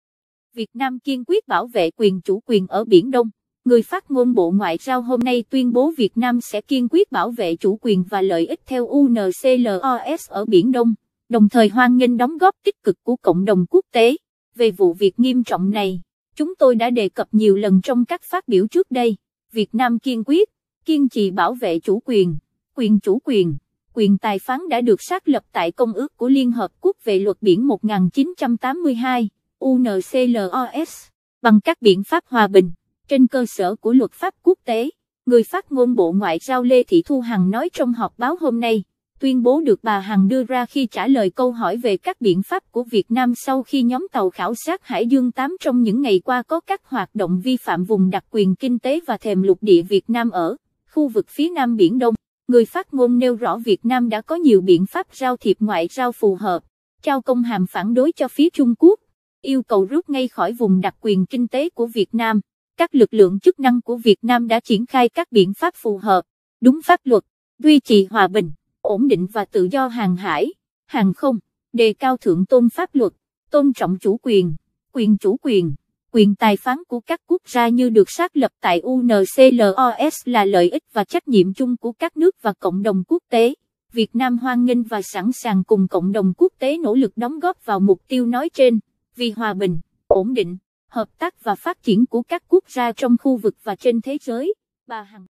Việt Nam kiên quyết bảo vệ quyền chủ quyền ở Biển Đông, BNG - 25/07/2019
Người phát ngôn Bộ Ngoại giao hôm nay tuyên bố Việt Nam sẽ kiên quyết bảo vệ chủ quyền và lợi ích theo UNCLOS ở Biển Đông, đồng thời hoan nghênh đóng góp tích cực của cộng đồng quốc tế.